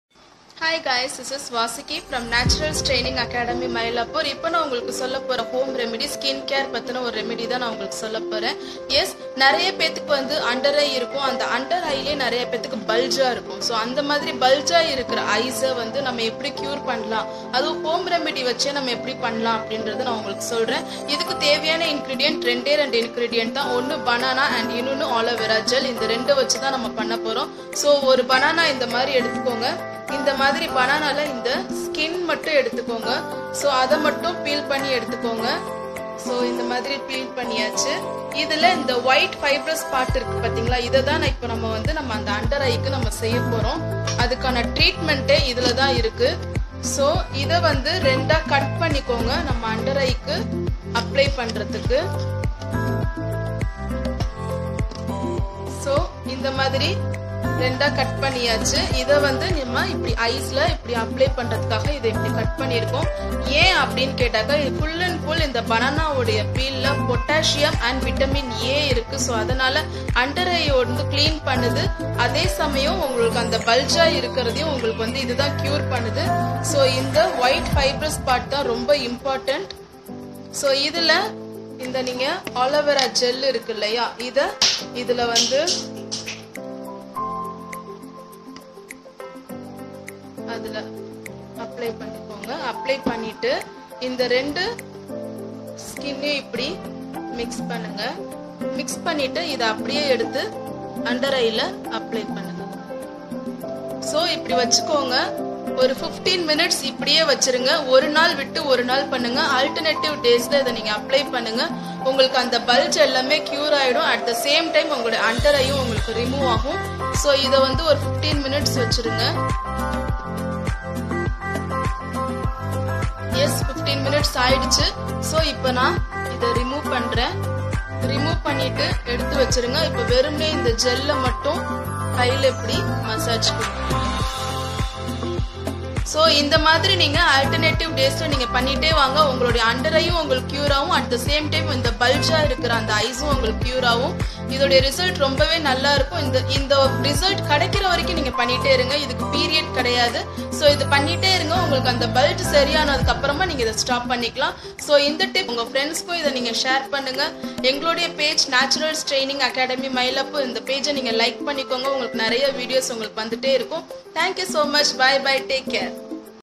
DIY UNDER EYE PUFFINESS...
Ingredients. ▪️Ripe Banana Peel ▪️Aloevera gel
Tear a banana peel into half and add some Aloevera gel over the white fibrous part.
Now rub the two slices together and place them on under eye area for some 15 minutes. Remove and massage the excess off the area. Do this every alternate day till you see the results.
You can store the other banana peels in refrigerator so that there is no wastage.
Banana peels are full of potassium and vitamin A that help in healing the under eyes and also cool down the area.
Do this alternative days. thank u